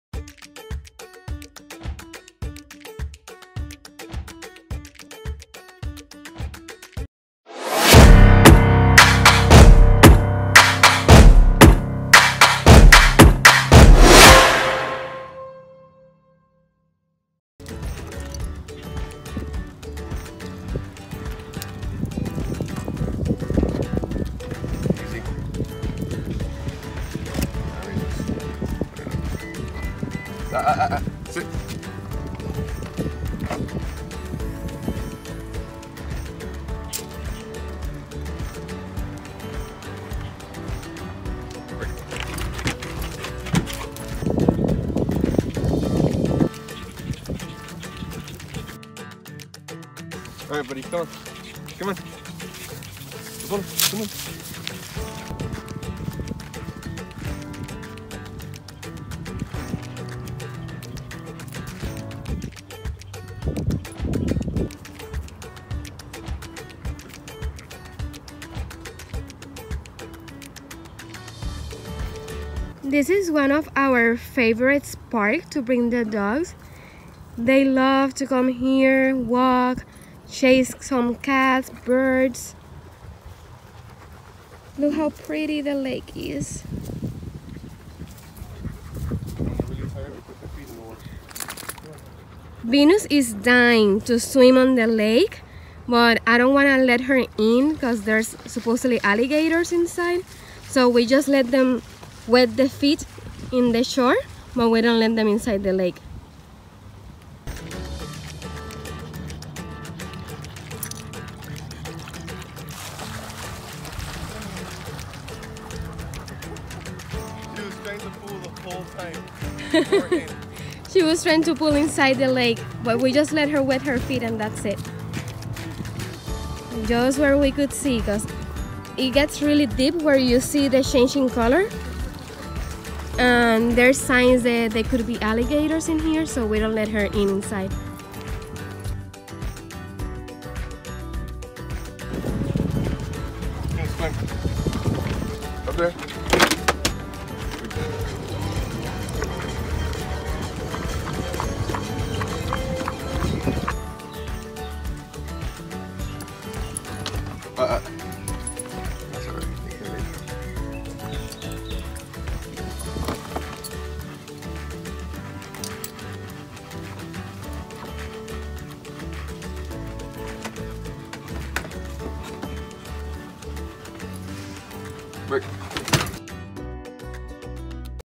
Taking my Malamutes to the lake
Venus & Apollo love going for walks and this is one of our favorite parks to go, the lake is so pretty and the dogs loves to see the ducks on the lake and sometimes chase a wild cat.

Gear on this video
iPhone X